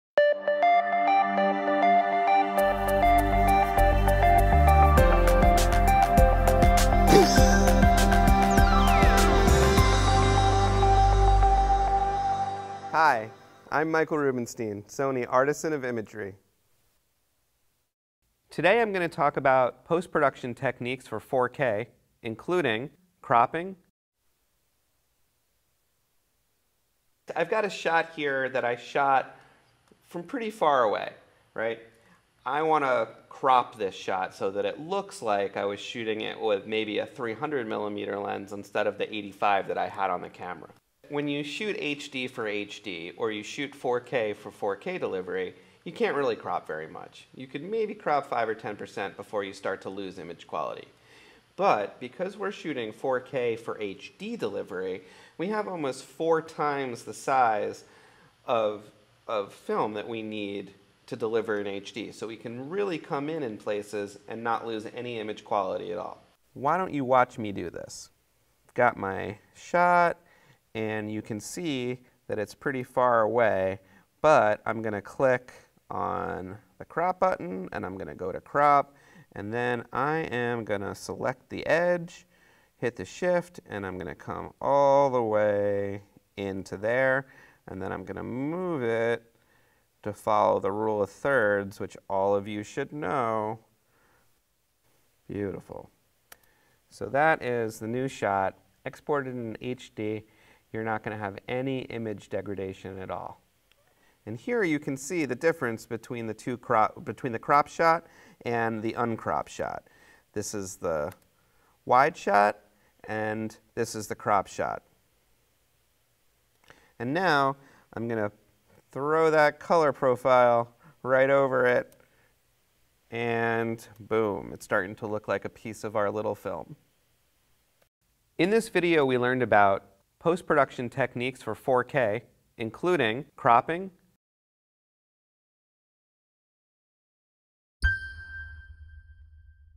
4K Post-Production Techniques: Cropping
Sony Artisan Michael Rubenstein shows how you can crop your 4K video footage for HD in post-production without losing image quality.